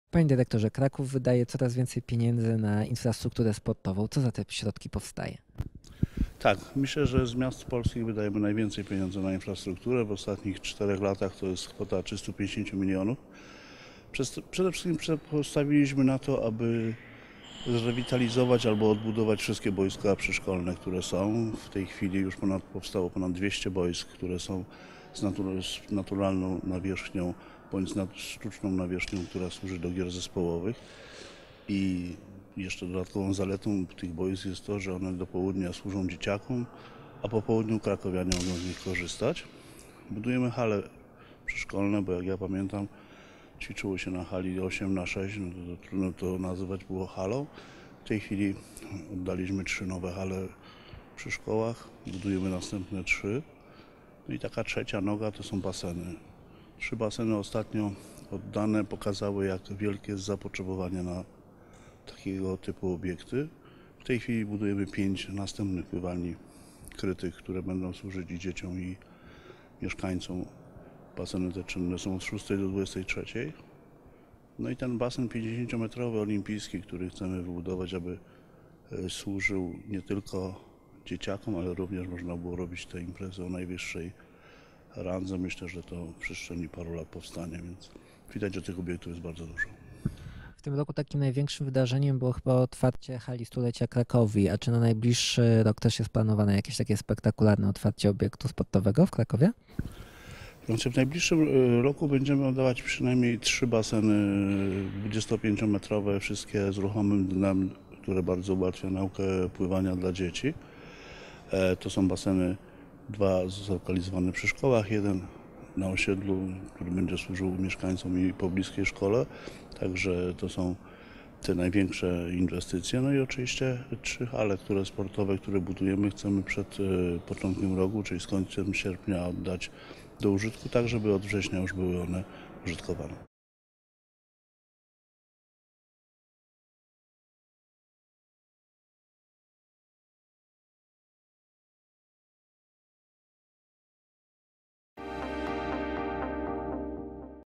Na co Kraków wydaje więcej niż inne miasta?
- Myślę, że z polskich miast wydajemy najwięcej na infrastrukturę sportową - powiedział Fakt24 Krzysztof Kowal, dyrektor Zarząd Infrastruktury Sportowej w Krakowie. Miejskie środki zostały wykorzystane m.in. na rewitalizację boisk przyszkolnych, budowę basenów i hal sporotwych.